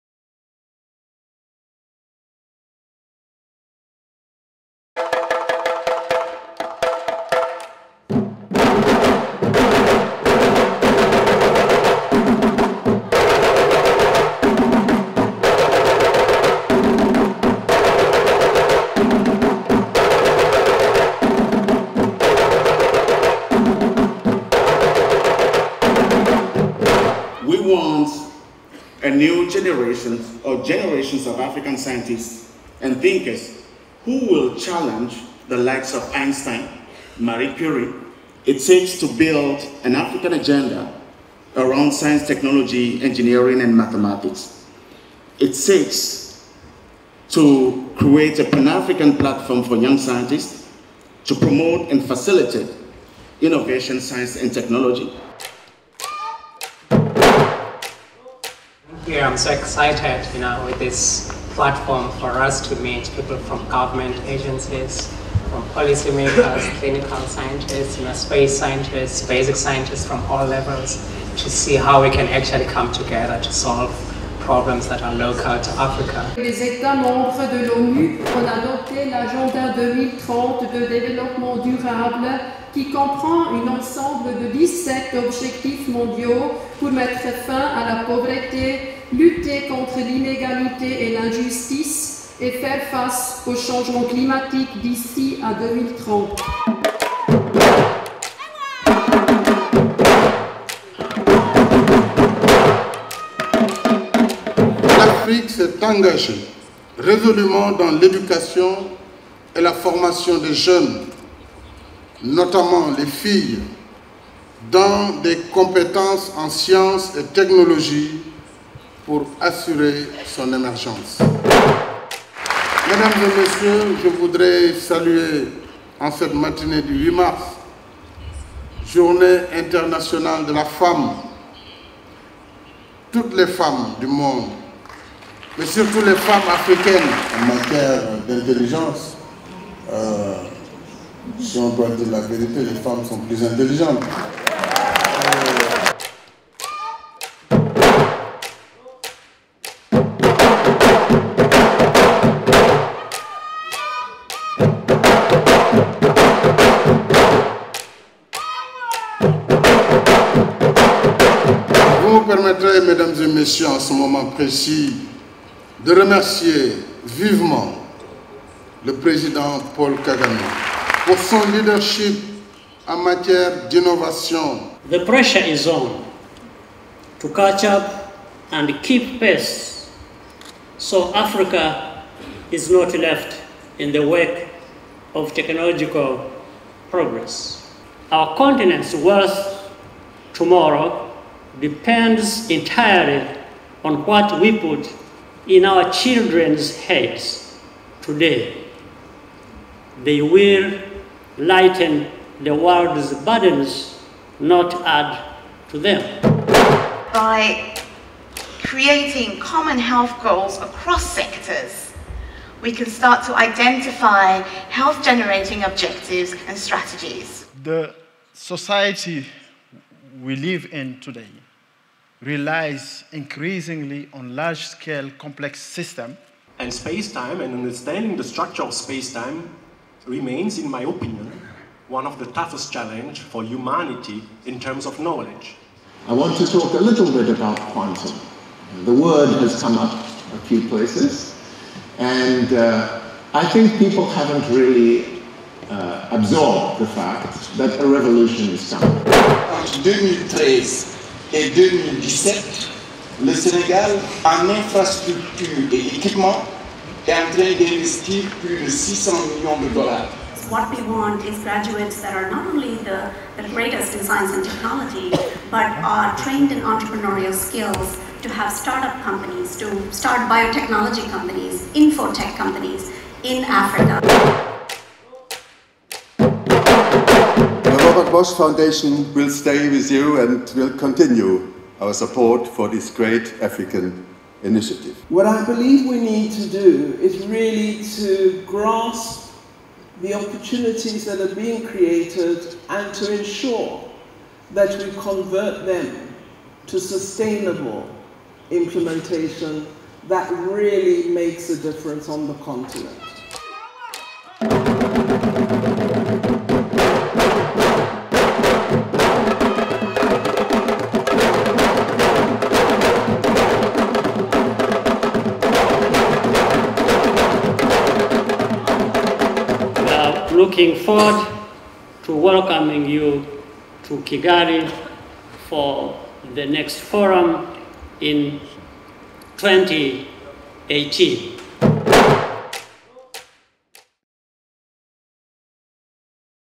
Highlights of NEF Global Gathering 2016 Day 1
Highlights of the Next Einstein Forum Global Gathering 2016 opening ceremony, Presidential Panel and day 1 plenaries and parallel sessions.